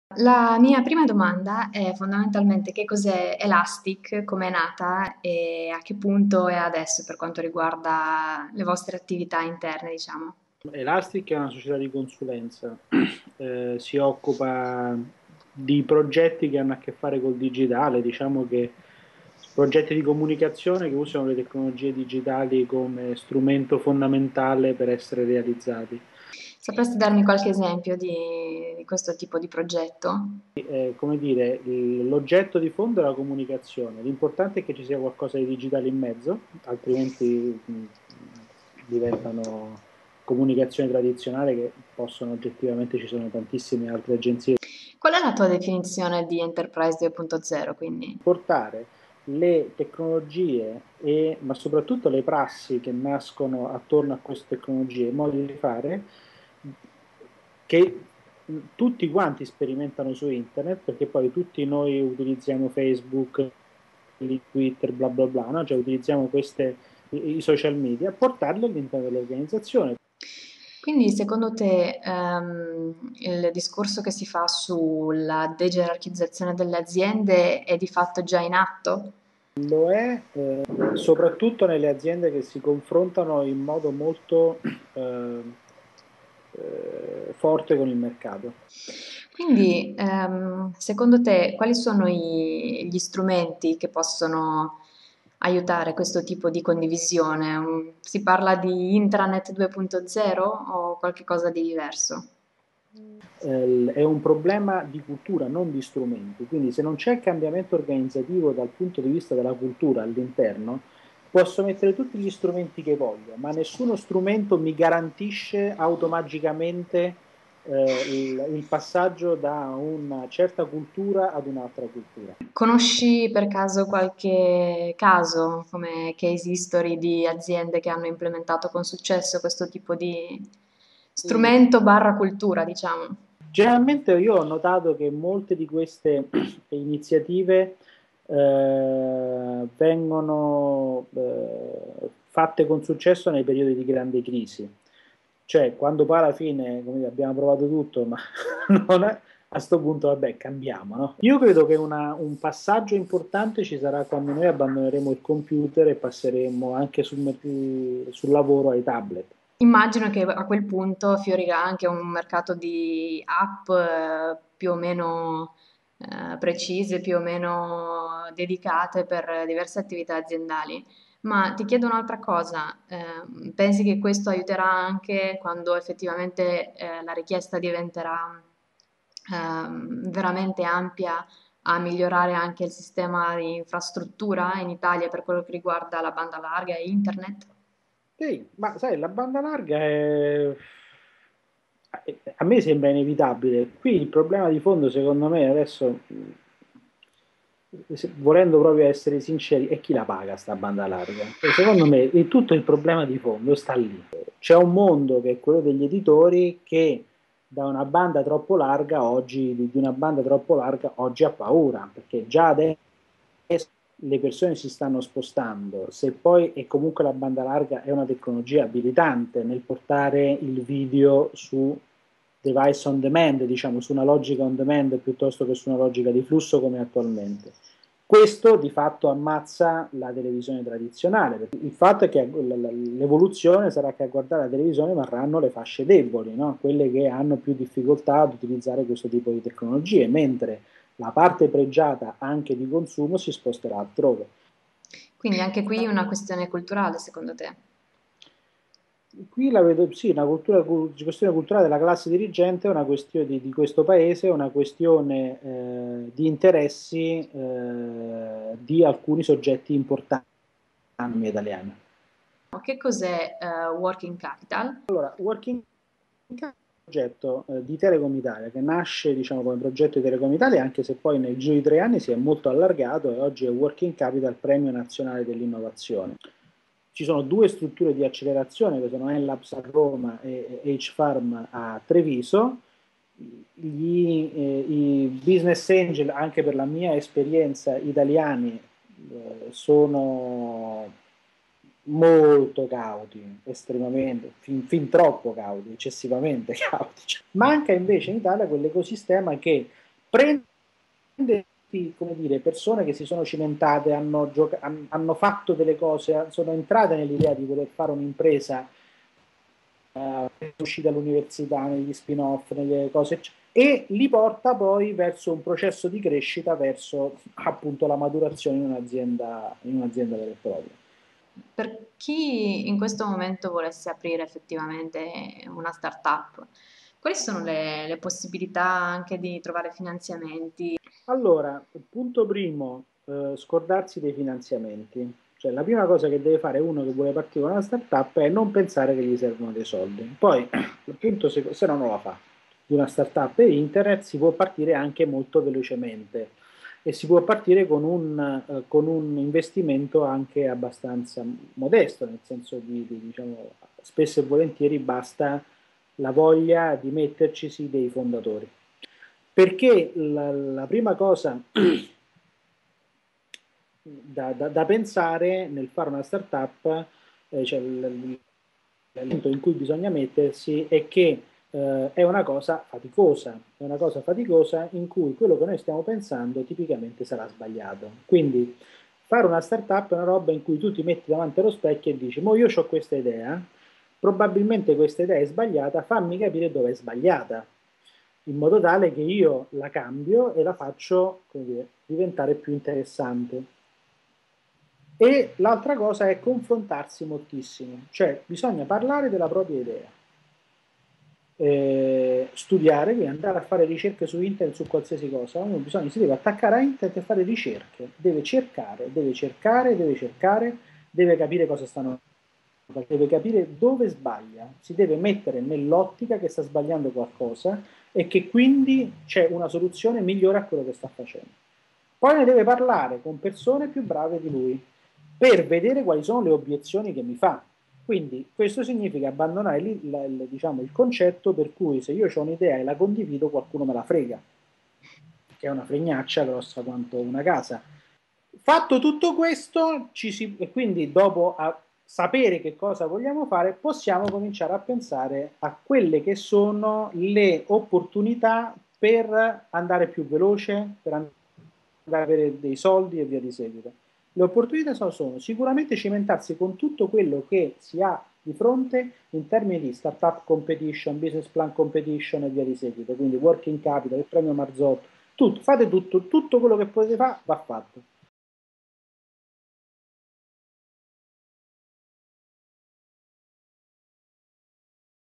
Intervistato.com | Nicola Mattina @nicolamattina in 7 minuti
ITA/ENG

Su Intervistato.com abbiamo avuto il piacere di intervistare Nicola Mattina, fondatore di Elastic ed esperto di social media e comunicazione corporate.

On Intervistato.com we had the pleasure of interviewing Nicola Mattina, founder of Elastic and social media and corporate communication expert.